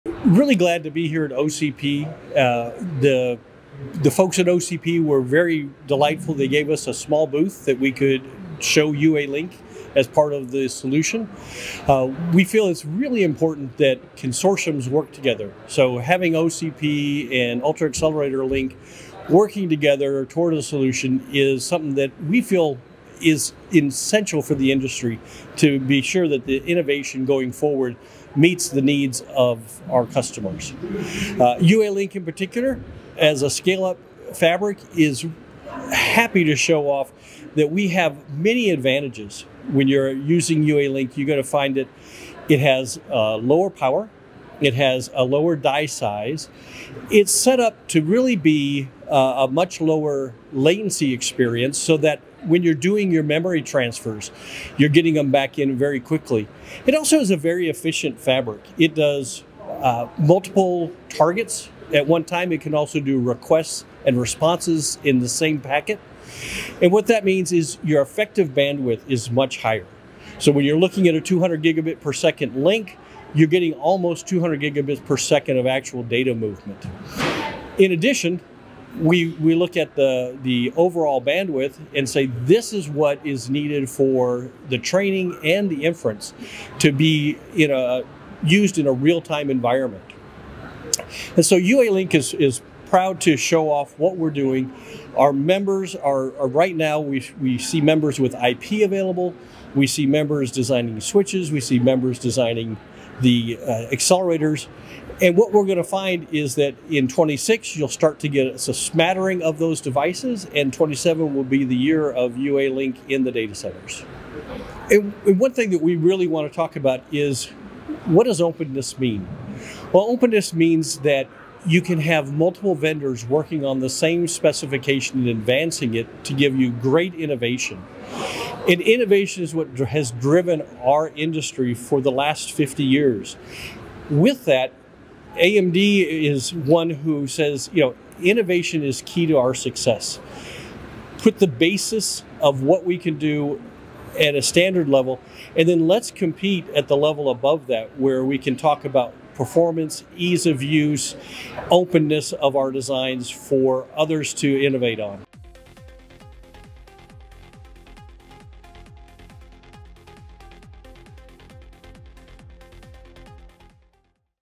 OCPSummit25: UALink for Scale-Up AI Interconnects
to get the attendee and sponsor perspectives from the show floor of the OCPGlobalSummit 2025

UALink Consortium Chairman Kurtis Bowman discusses the consortium's collaboration with the Open Compute Project and reveals key technical advantages of the UALink interconnect technology. Bowman outlines how UALink delivers superior performance through lower power consumption, reduced latency, and enhanced bandwidth efficiency compared to existing solutions. He provides insights into the growing member ecosystem and shares the consortium's roadmap for market deployment, while explaining the strategic philosophy behind open standards in accelerator interconnect technology.

- How UALink's technical architecture achieves lower power and latency than competing solutions
- The bandwidth efficiency advantages that enable near-theoretical data transfer rates
- Timeline for UALink adoption across data centers and member product availability
- Why consortium collaboration with organizations like OCP drives industry innovation
- AMD's competitive strategy around open standards versus proprietary approaches
- The role of openness in fostering multi-vendor innovation and market advancement

📚 CHAPTERS:
0:00:00 - Introduction and OCP Partnership
0:00:24 - UALink Technical Advantages and Performance Benefits
0:01:19 - Bandwidth Efficiency and Real-Time Applications
0:01:33 - Member Ecosystem and Market Timeline
0:01:58 - The Philosophy of Openness in Technology Standards
0:02:21 - AMD's Innovation Strategy and Competitive Approach